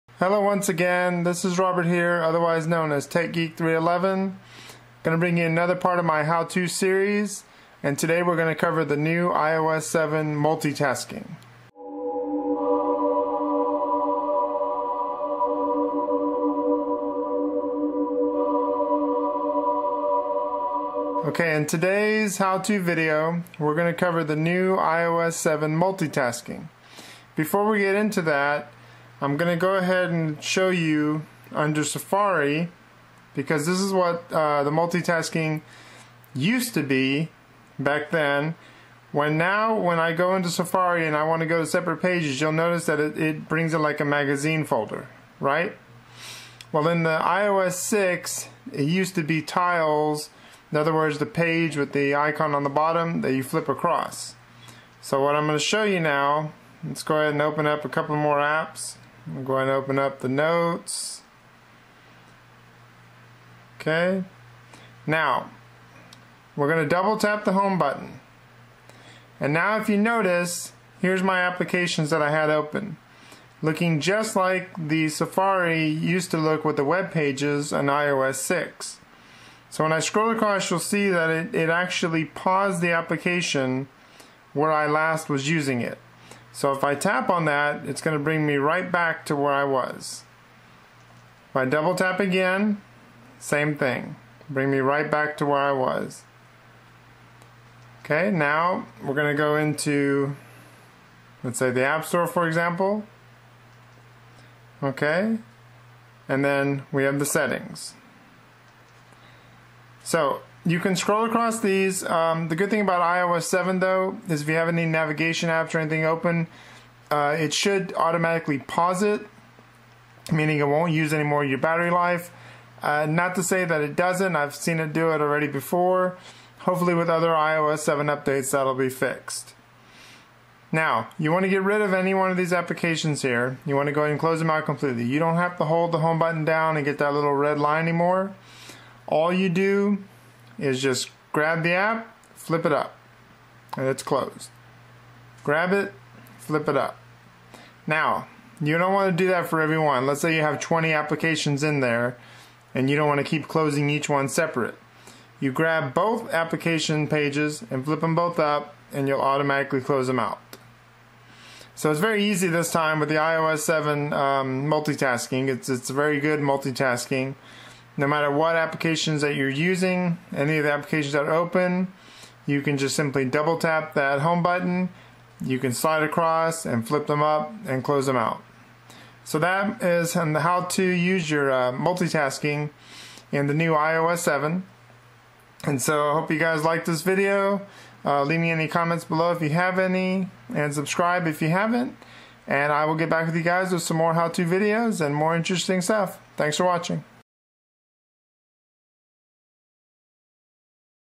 HOW TO use IOS 7 Multitasking
How To use iOS 7 new multitasking features. The new ios 7 multitasking features tiles of your currently used apps, each app is paused on the last page that you used. To remove apps from the new multitasking all you have to do is just swipe the app upwards and it'll close out the app completely. The new IOS 7 multitasking is very similar to the IOS 6 Safari app when you are switching websites.
The best part about the new IOS 7 multitasking is that when an app is open in the background, it no longer uses up the iPhones battery, but instead it pauses where the app was currently left off until you either resume the app or close it out completely.

Add me on Snapchat! Username: techgeek311